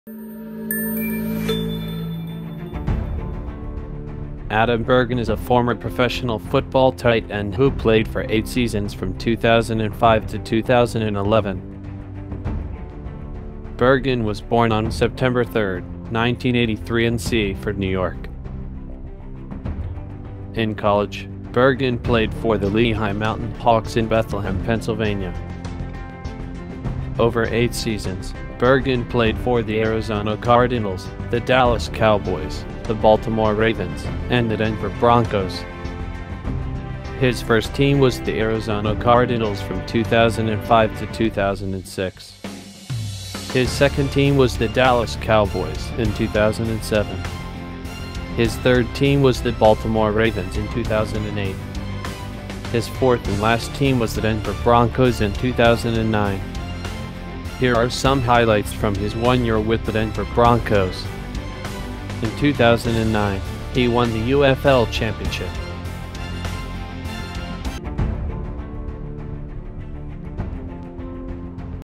Adam Bergen: Football Tight End
Adam Bergen is a former professional football tight end who played for 8 seasons from 2005 to 2011.

In college, Bergen played for the Lehigh Mountain Hawks in Bethlehem Pennsylvania.

Professionally, Bergen played for the Arizona Cardinals, the Dallas Cowboys, the Baltimore Ravens, and the Denver Broncos.

In 2009, he won the UFL championship.